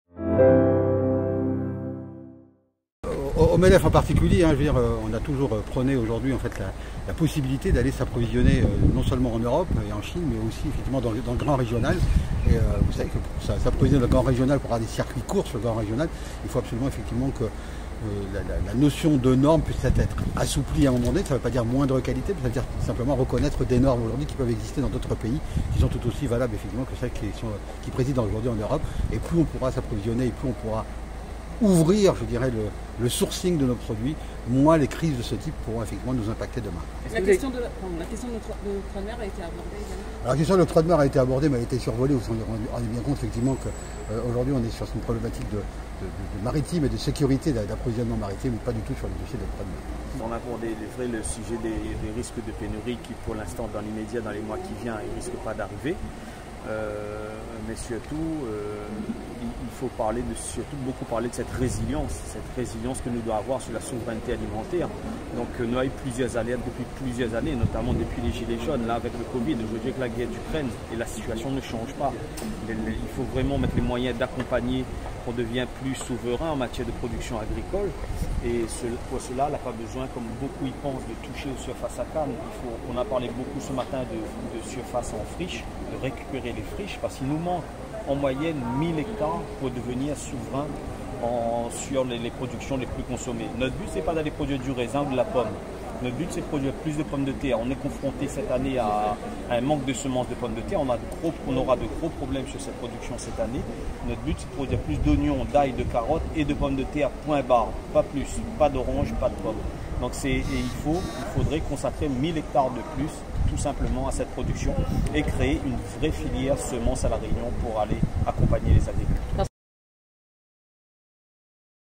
Guerre en Ukraine et ses conséquences à La Réunion : la réaction des acteurs économiques
Didier Fauchard, président du MEDEF Réunion et Frédéric Vienne, président de la chambre d'agriculture, livrent leur point de vue après la tenue ce 1er avril d'une réunion à la Région Réunion sur les conséquences économiques de la guerre en Ukraine.